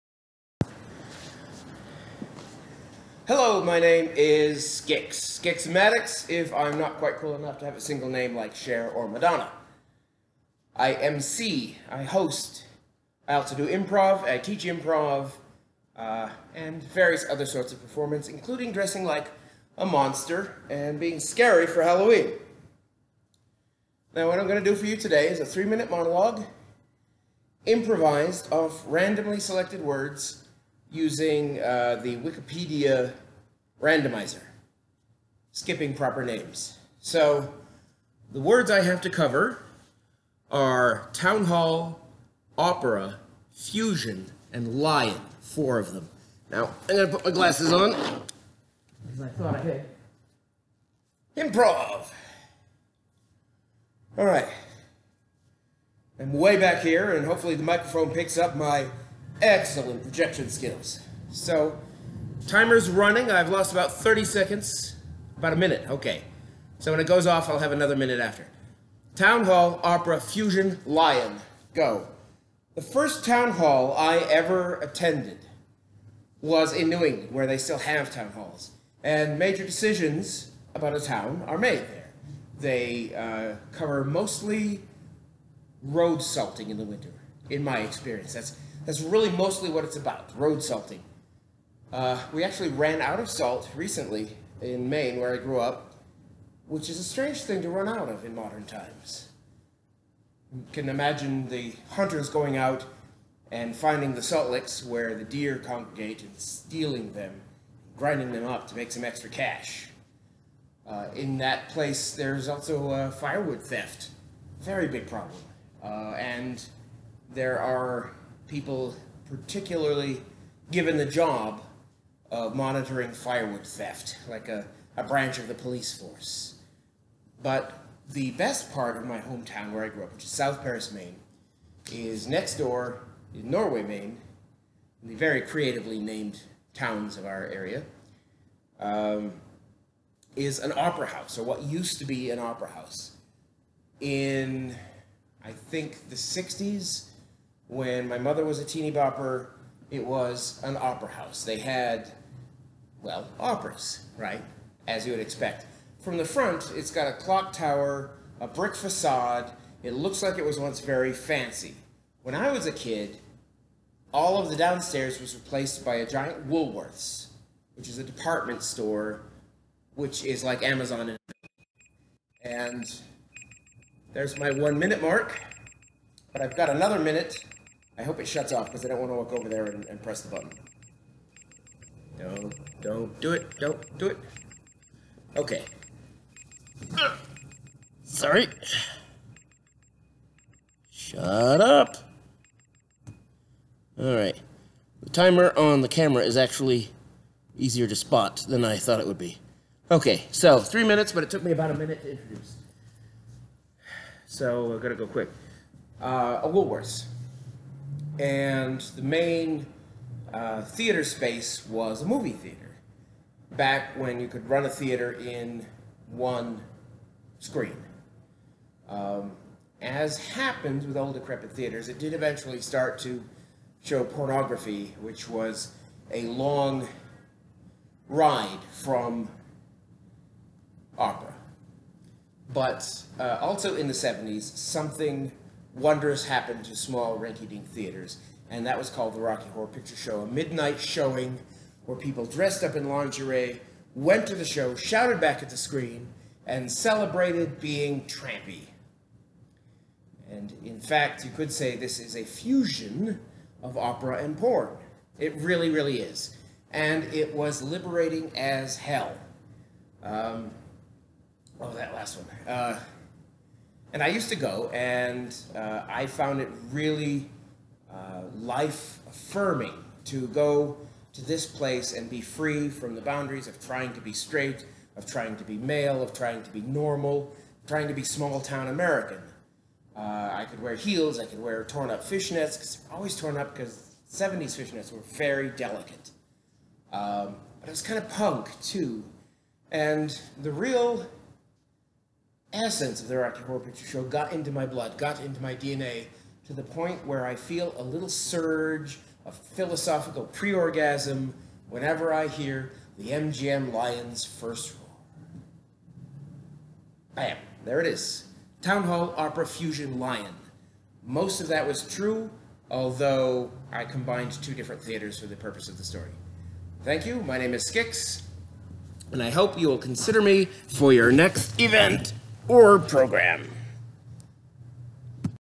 for your consideration
3 minutes improvised, using the randomly-generated prompts, Town Hall, opera, fusion and lion.